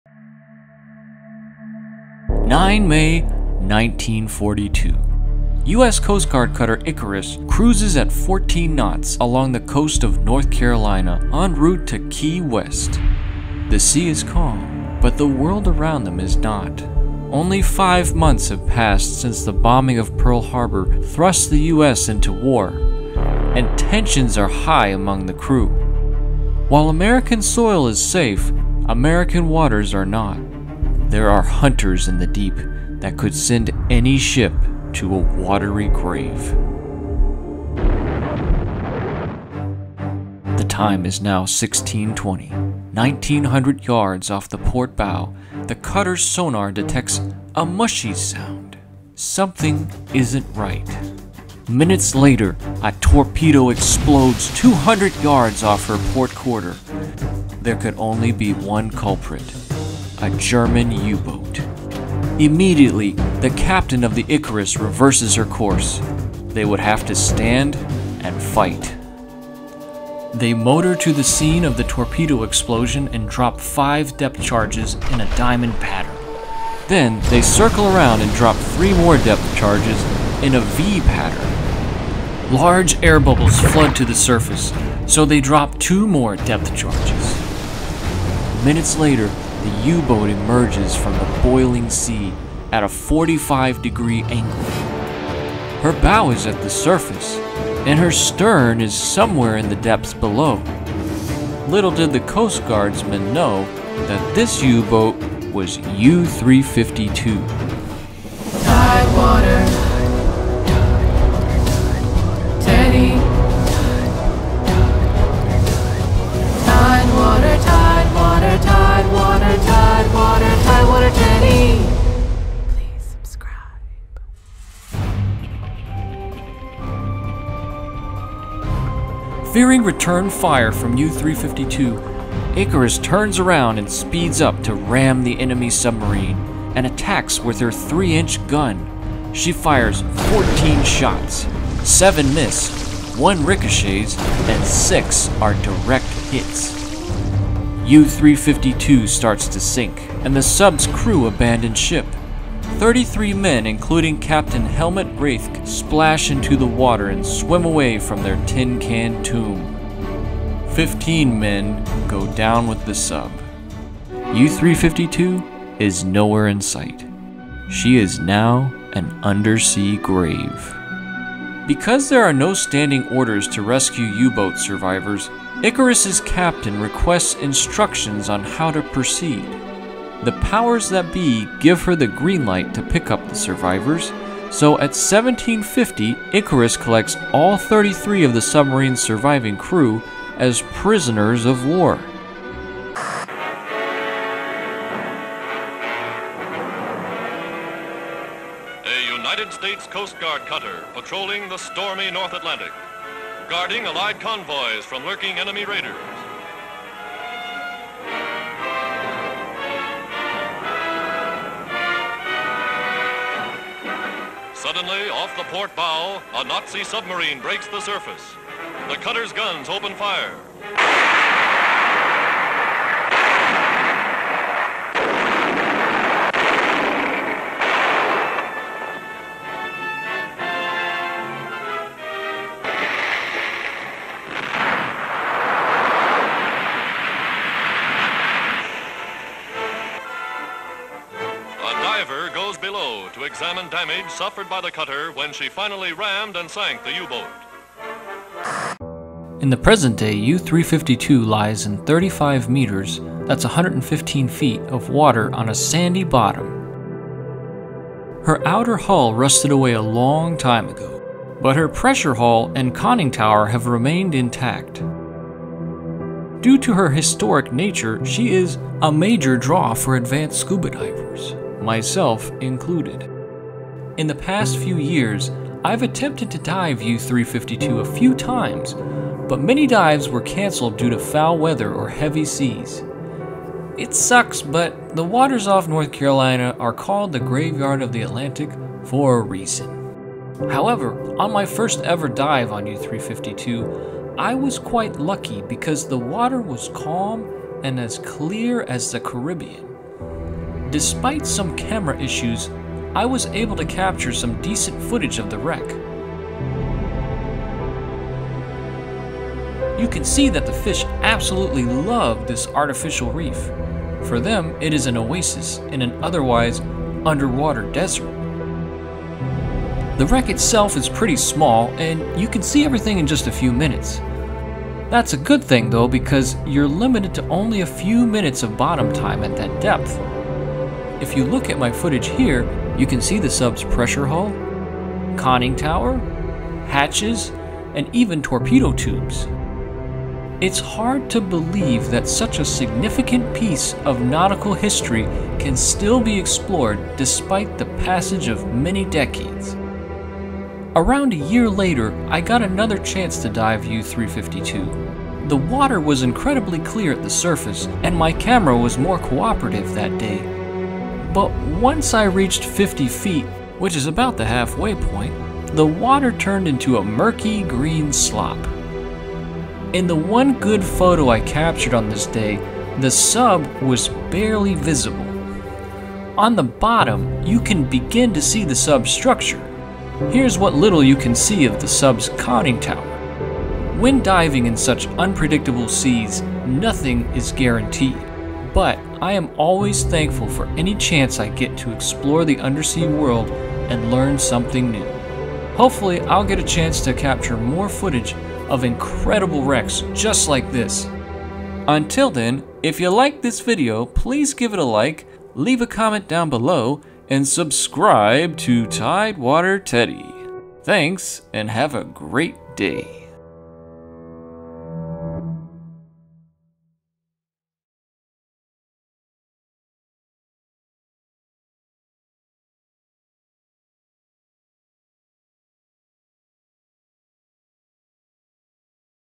Diving on the Wreck of a WW2 Submarine - U-352
During World War II, German submarines, known as U-boats, terrorized the North Atlantic. These Nazi subs were lethal to navies and merchant fleets, sinking hundreds of ships and thousands of tons of cargo. In May 1942, a U-boat known as U-352 faced off against the US Coast Guard Cutter Icarus. In the end, U-352 lost and sank to the bottom of the sea.

Today, she lies in 35 meters (115 feet) of water off the coast of North Carolina. Due to her historic nature, she is (understandably) a major draw for advanced scuba divers, myself included. Learn the history of U-352 and join me on an undersea adventure to explore her wreck.

0:00 USCGC Icarus is attacked
02:07 U-352 is sunk
03:13 Patrol Boat Sinks Nazi U-Boat in North Atlantic
04:08 The U-352 wreck today
05:50 Diving in the murk
06:23 More wrecks?